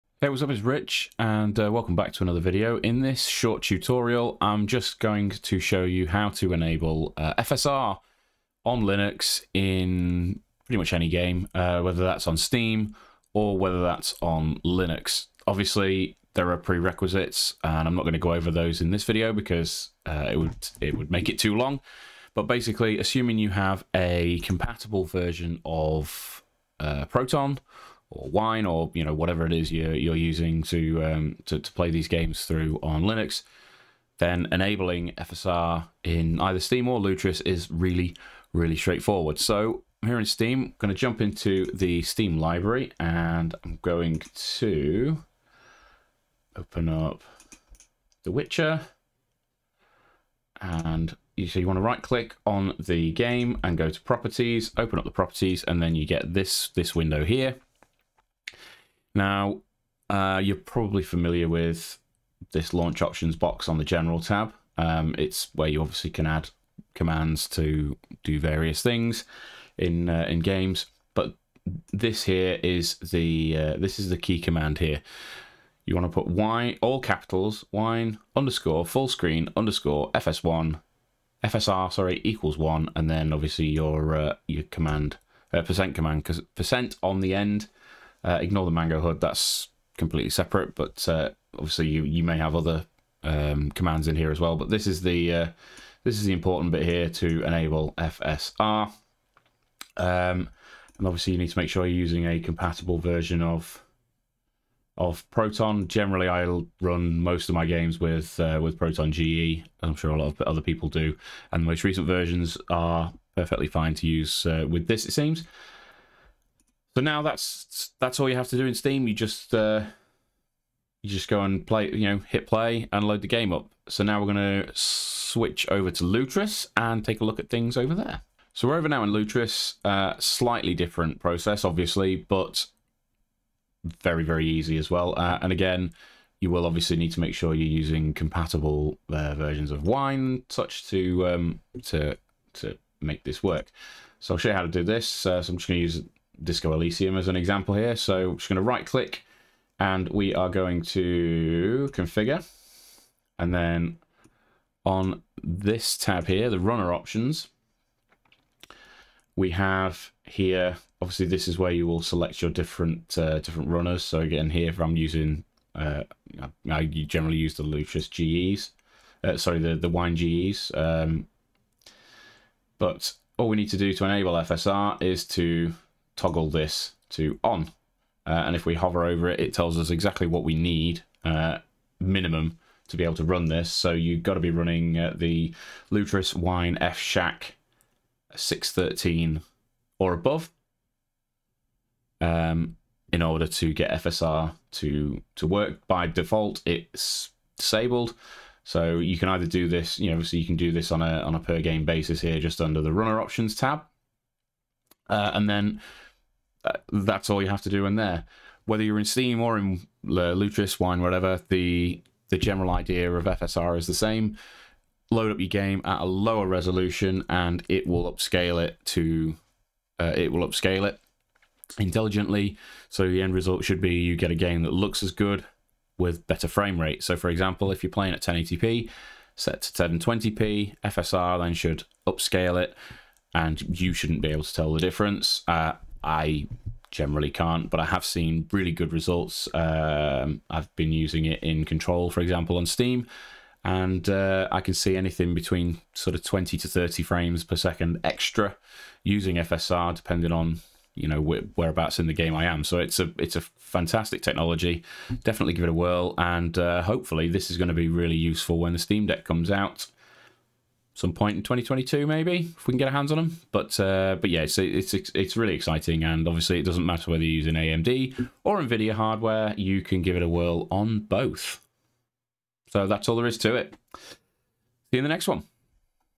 How to enable AMD FidelityFX Super Resolution (FSR) on Linux in Steam and Lutris
A quick demonstration on how to enable FSR support in any game on Steam and Lutris on Linux.